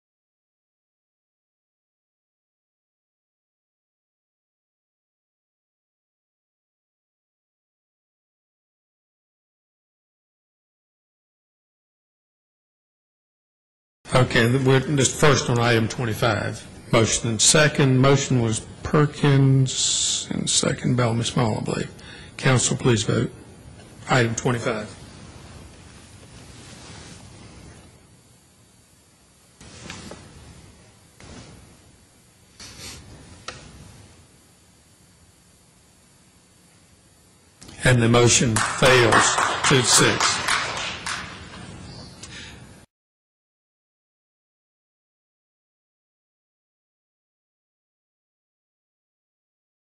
Greensboro City Council with a Conservative Vote and Applause from Audience
THe Greensboro City Council on 1-4-2011 had a 6-2 vote on this item,  25. Resolution approving Memorandum of Understanding (MOU) for the Triad Region Intelligent Transportation System (ITS) Deployment Plan -- and Implementation of Phase 4 (Real Time Passenger Information System). Then you see after the conservative a applause from the audience